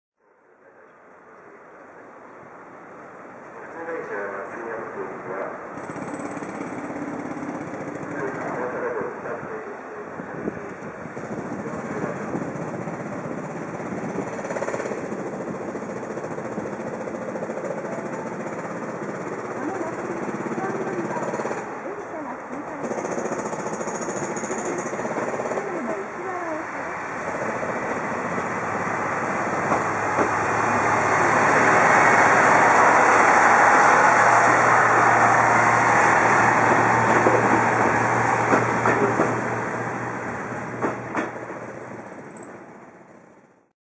城東貨物線 キヤ141系 キヤ検 鴫野（往路） 2011.01.26
城東貨物線（片町線）
キヤ141系 第1編成 ドクターWEST （往路）
鴫野通過
2011年1月26日撮影